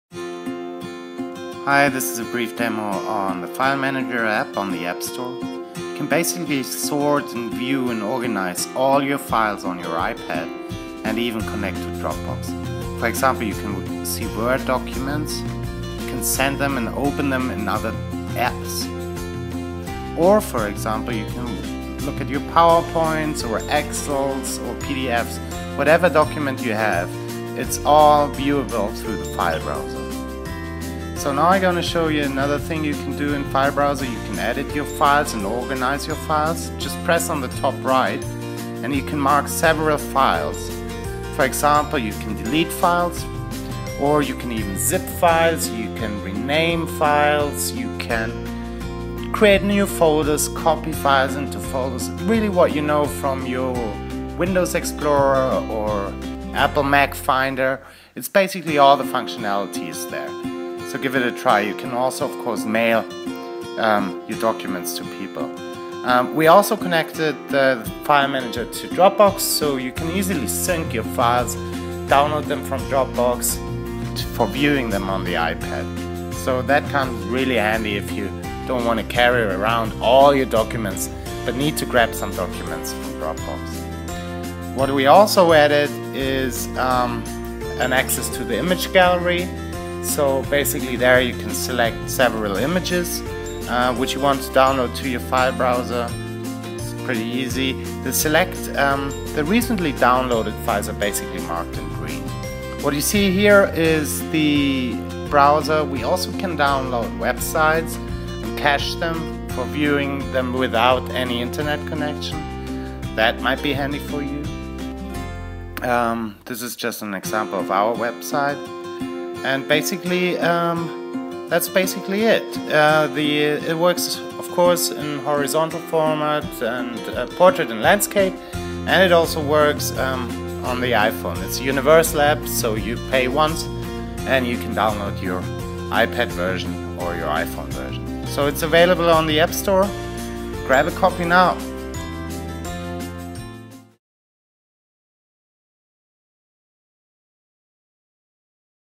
File Manager App for iPad and iPhone
File Manager is the ultimate app to download, organize and view all your files on your iPhone or iPad.

It is like the Windows Explorer on your PC or the Finder on your Mac. Always have your important files with you.

DOWNLOAD CONTENT

Getting your documents onto your iPad is really easy. You can download your files from your computer using iTunes or import them from Dropbox. You can also open files from Email or browse the web for content to download. Import options are:

-Internet
-Dropbox
-From other apps

ORGANIZE DOCUMENTS

The intuitive user interface is amazing and makes organizing your content just a breeze. The app combines traditional file browsing with a truly touch enabled interface. In detail the app allows you to:

-Print
-Zip and unzip
-Copy files
-Move documents
-Create new folders
-Rename files
-Email files

VIEW FILES

File Manager has a robust viewer for documents such as PDF files, MS Office documents. You can also view images, play your mp3s or watch videos with the in-built players. In total File Manager supports the following formats:

-Powerpoint
-Excel
-Word
-Keynote
-Numbers
-Pages
-Images
-Web Files
-Audio
-Zip Files

File Manager is an universal app. You can download File Manager for the iPhone and the iPad paying once only. Always have your important files at your fingertips.